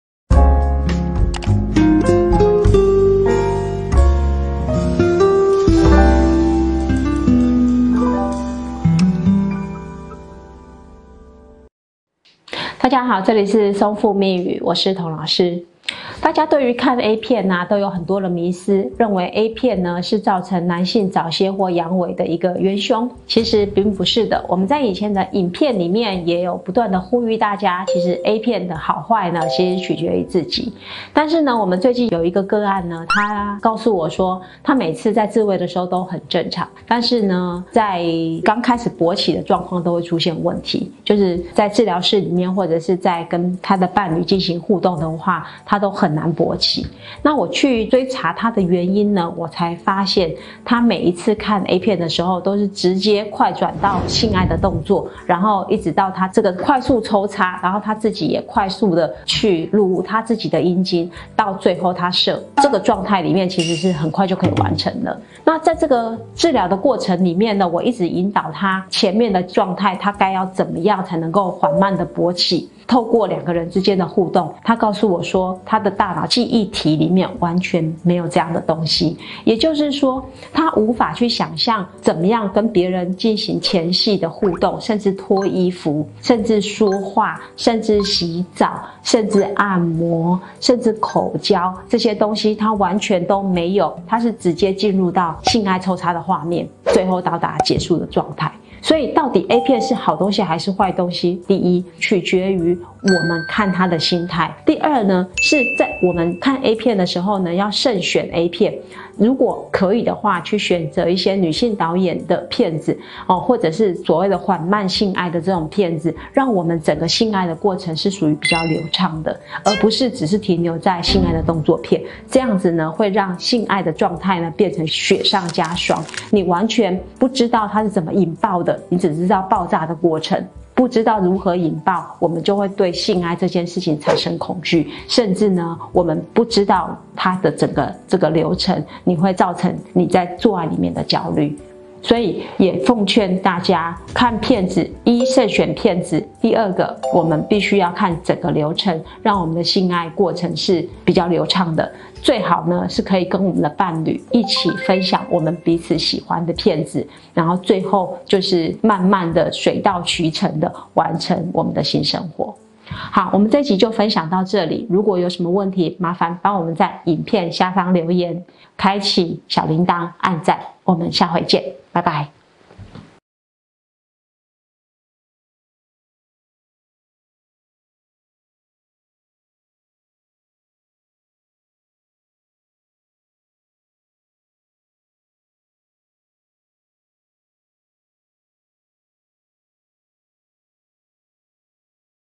揭開A片真相: 太早射精的真正元凶到底是什麼？
很多人都認為A片是造成男性陽痿或早洩的主要原因，但實際上，這種觀點是錯誤的。事實上，A片對於性健康的影響是很有限的，關鍵在於我們自己的選擇和觀念。
首先，我們需要慎選A片，不要只是盲目地追求刺激和快感，而是要尋找符合自己口味和喜好的影片，這樣才能真正地享受觀看的過程。
其次，我們也需要注意整個性愛過程中的流程，不要讓自己過於著急或焦慮，而是要慢慢地享受這個過程，這樣才能讓性生活更加流暢和愉悅。
最後，我們可以與伴侶一起分享我們喜歡的A片，這樣不僅可以增進彼此的感情，也可以更好地了解對方的需求和喜好，進而建立更加健康和美滿的性生活。
總之，A片並不是造成陽痿或早洩的主要元兇，而是我們自己的選擇和觀念決定了它對我們的影響。只要我們採取正確的態度和方法，就能夠享受健康、快樂和愉悅的性生活。
-- 合作專欄 --
【東森新聞】 性治療正夯! 治療室真實性愛找問題
【澎湃新聞】被忽視的愛與性：從“ 性健康管理師 ” 的課程筆記說起